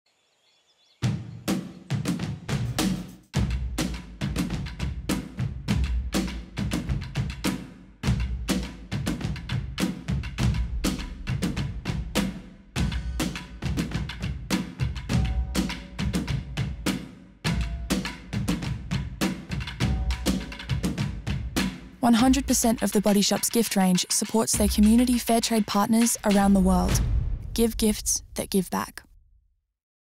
WHAT ARE YOU SHOPPING FOR?
This Christmas we are celebrating our Community Fair Trade partners around the world, who have lovingly enriched our gift collection with natural ingredients and/or handcrafted, sustainable packaging. It's important to shop consciously, which includes knowing who made your gift, how it was sourced and how they were treated.